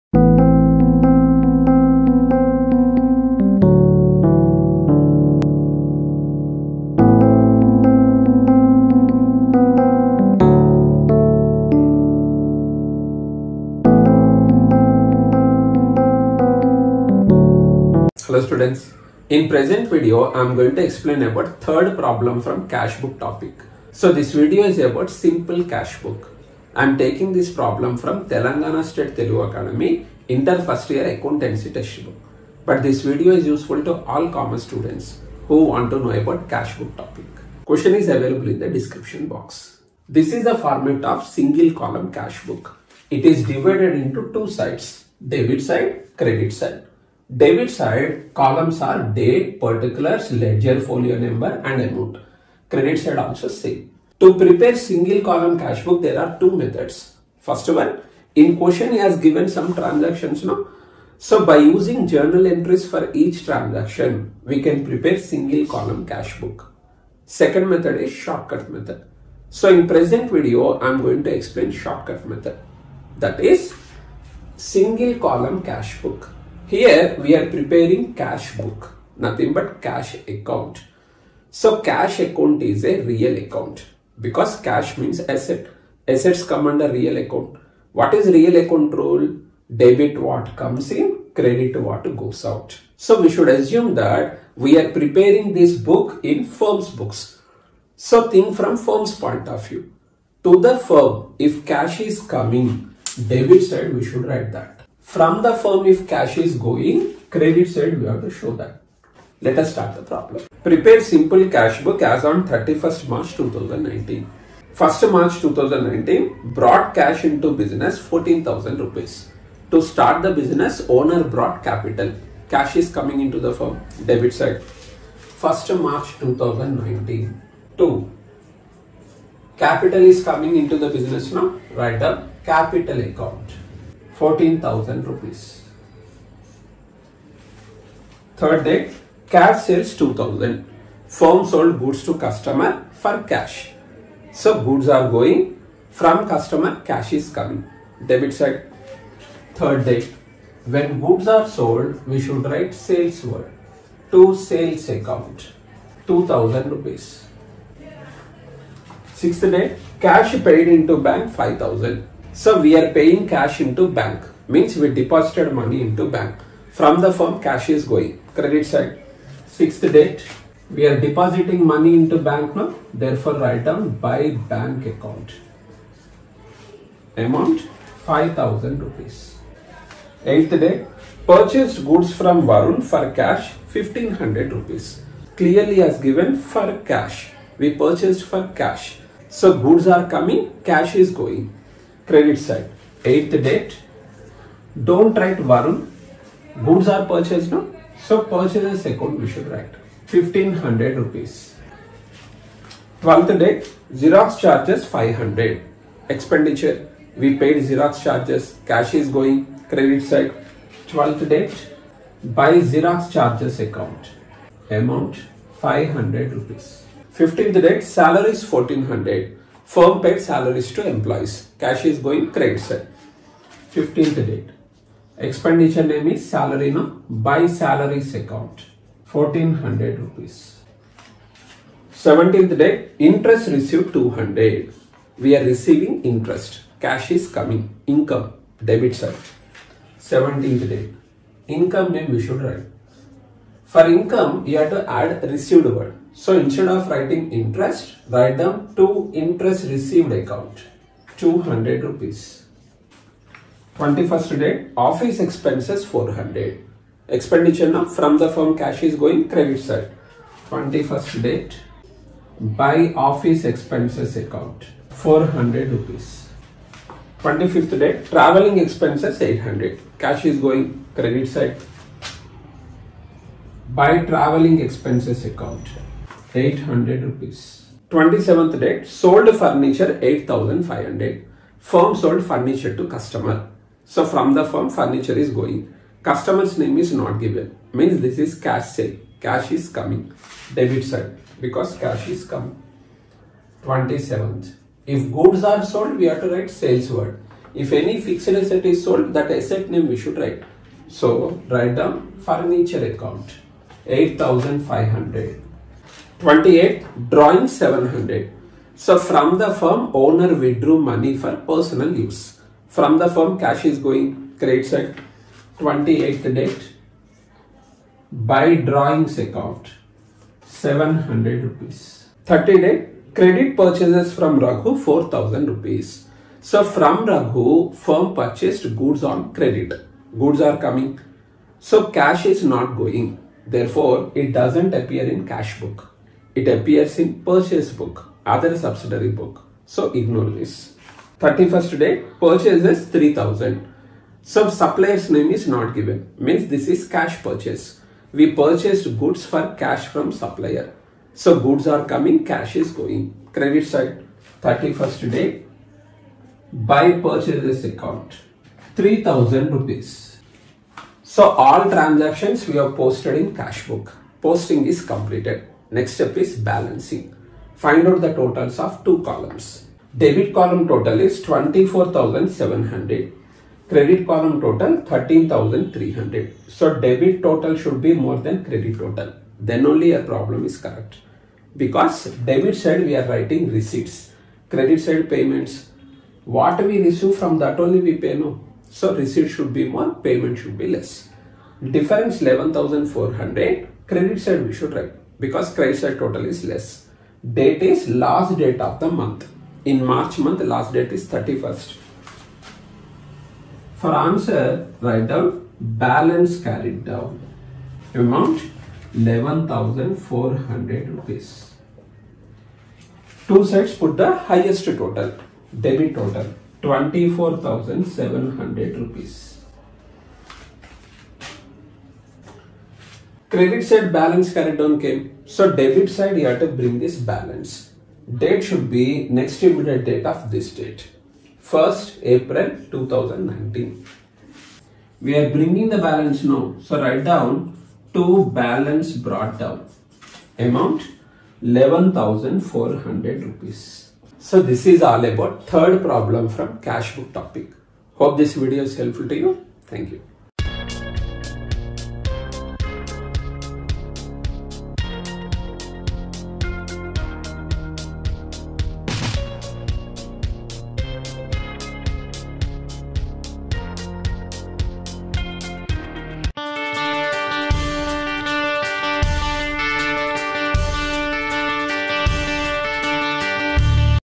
Simple Cash Book || Problem no - 3
3. Prepare Simple cash book as on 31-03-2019.
2019                                                                                Rs
Mar 1st   Brought cash into business                   14.000
3rd           Cash Sales                                                  2.000
6th           Cash paid into bank                                   5.000
8th           Purchased goods from Varun for cash  1.500
12th         Xerox charges                                               500
15th         Salaries                                                       1,400
17th         Interest received                                           200
21st        Office expenses                                             400
25th        Travelling expenses                                      800
27th        Sold furniture                                              8,500
28th        Drawings                                                        700
30th        Credit purchases from Raghu                 4,000
31st        Purchases                                                   3,000